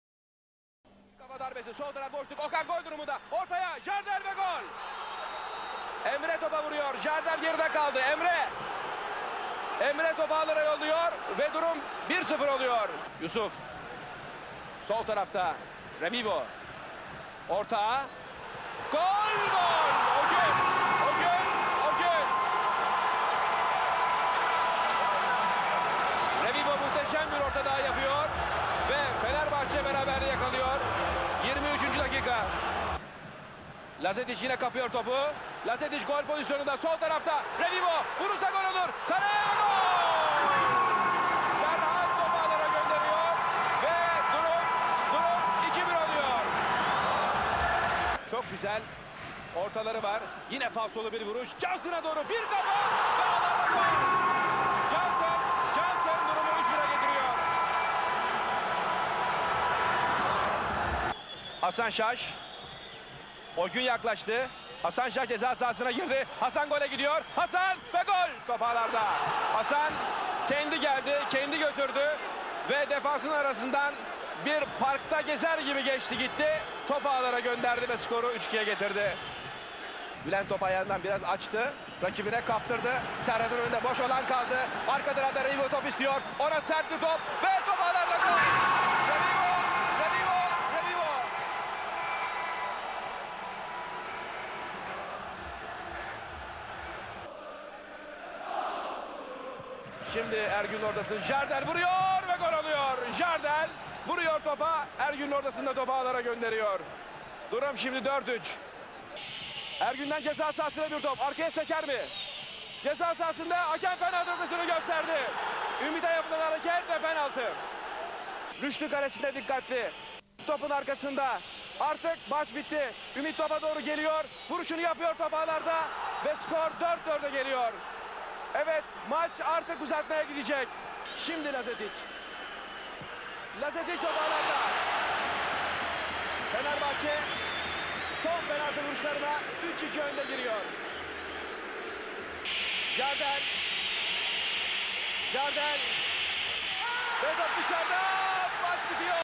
Fenerbahçe 4-4 Galatasaray (07.02.2001) (Türkiye Kupası Yarı Final)
2000/2001 sezonu Türkiye Kupası Yarı Final maçında Kadıköy'de iki takımın da alkışlandığı maç 4-4 bitiyor ve seri penaltılarla 3-2 kazanan Fenerbahçe finale çıkıyor.
Karşılaşmadaki 8 gol de farklı oyunculardan gelirken, penaltıyı kaçıran isim Jardel oluyor.

Stad: Şükrü Saraçoğlu
Hakem: Metin Tokat
Fenerbahçe Goller: Ogün Temizkanoğlu (22'), Serhat Akın (29'), Samuel Johnson (37') ve Haim Revivo (66');
Galatasaray Goller: Emre Belözoğlu (5'), Hasan Şaş (52'), Jardel (84'), Ümit Davala (90'p)

Fenerbahçe İlk 11:
Rüştü Reçber
Ogün Temizkanoğlu
Mustafa Doğan
Zoran Mirkovic
Abdullah Ercan
Samuel Johnson
Mirko Mirkovic
Nikola Lazetic
Yusuf Şimşek
Serhat Akın
Haim Revivo
T.D.: Mustafa Denizli

Galatasaray İlk 11:
Claudio Taffarel
Bülent Korkmaz
Gheorghe Popescu
Ümit Davala
Capone
Ergün Penbe
Emre Belözoğlu
Okan Buruk
Suat Kaya
Hasan Şaş
Jardel
T.D.: Mircea Lucescu